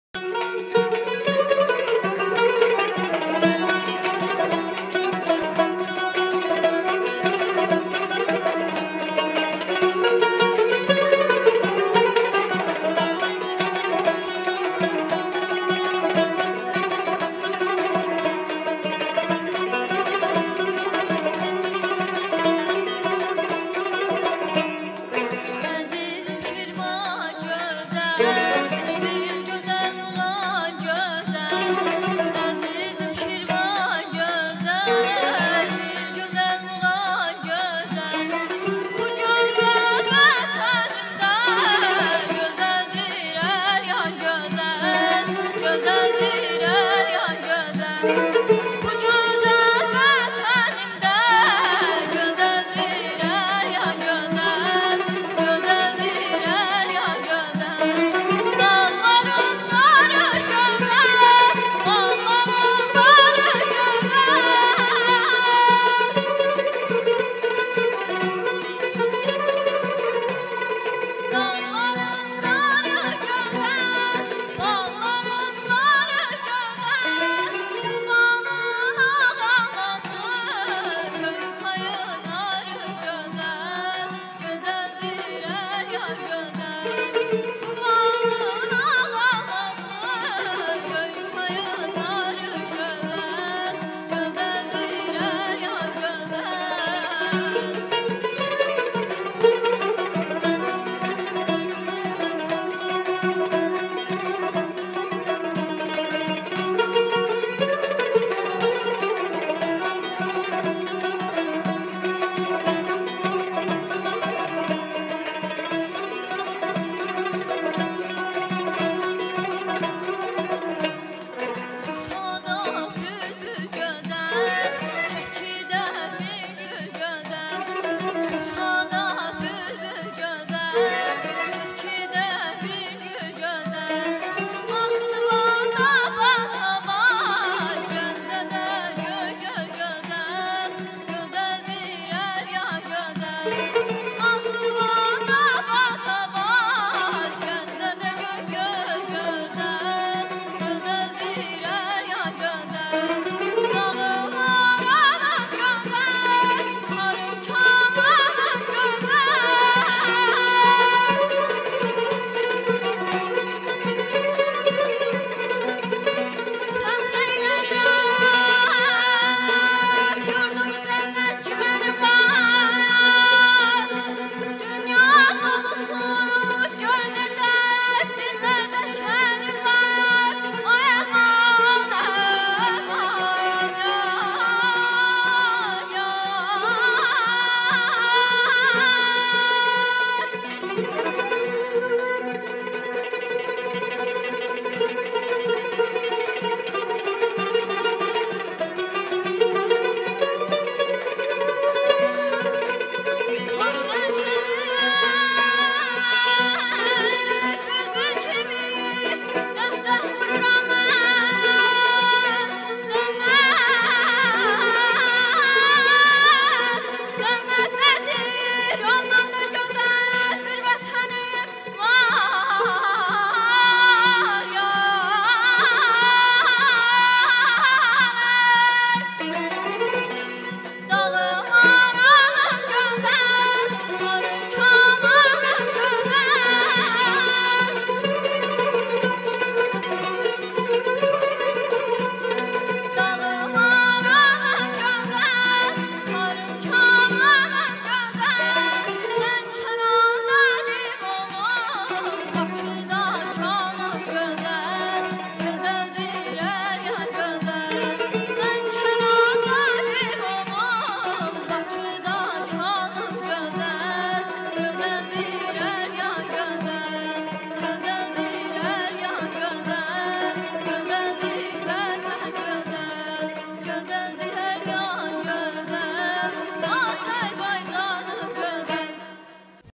12 Məmmədova Mələk
II Международный музыкальный конкурс
‘’CA MELODY’’
Турция
(по видеозаписям)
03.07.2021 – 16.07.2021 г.